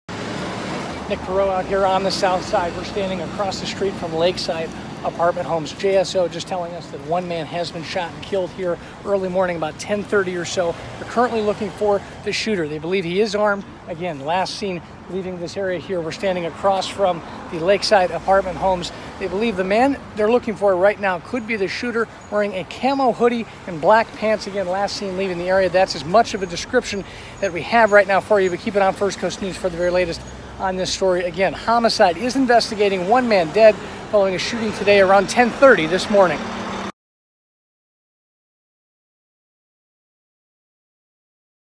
Man dies in parking lot of Lakeside Apartment Homes after being shot
Police are looking for a man with a camouflage hoodie and black pants after a man was shot and killed in the parking lot of Lakeside Apartment Homes.